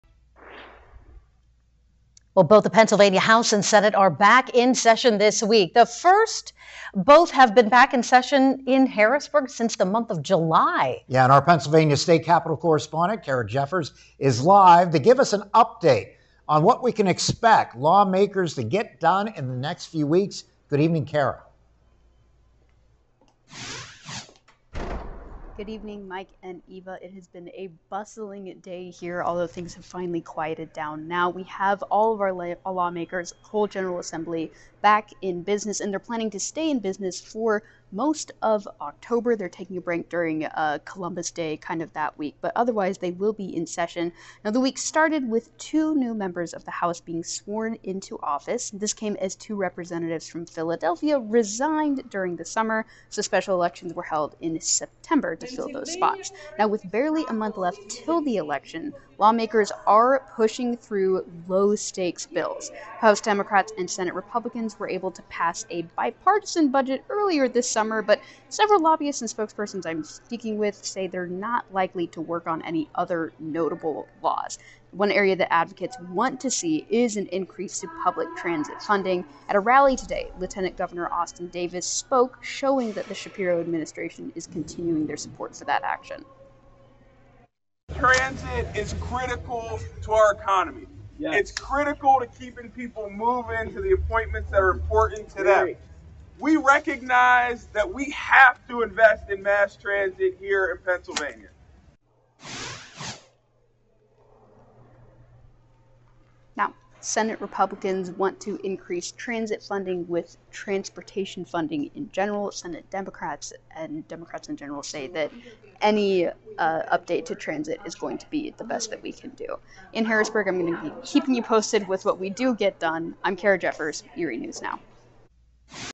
PA Lawmakers Back in Harrisburg
What to expect legislatively from Pennsylvania lawmakers this month.